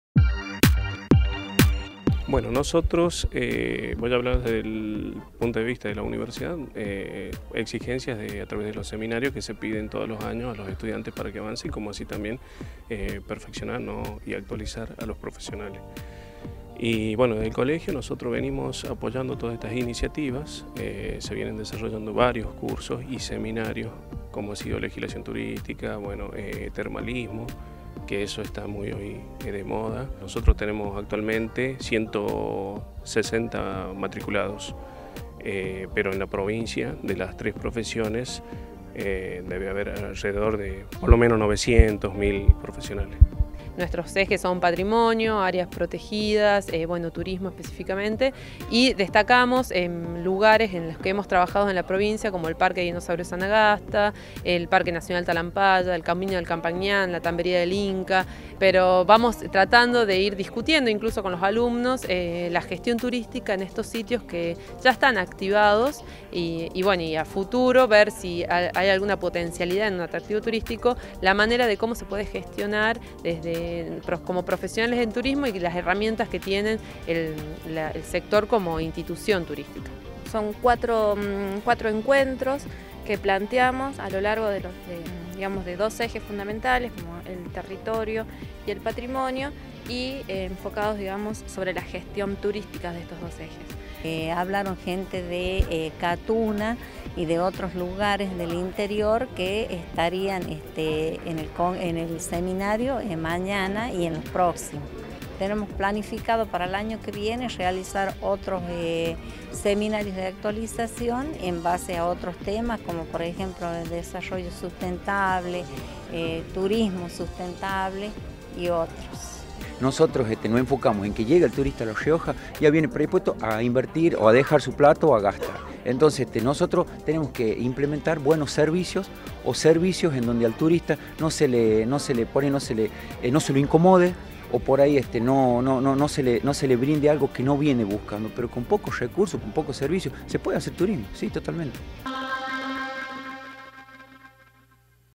U Noticias- Actualización en gestión turística: Capacitan a estudiantes y graduados de la carrera
La capacitación tendrá como temática de estudio y análisis la gestión turística en relación al territorio y el patrimonio.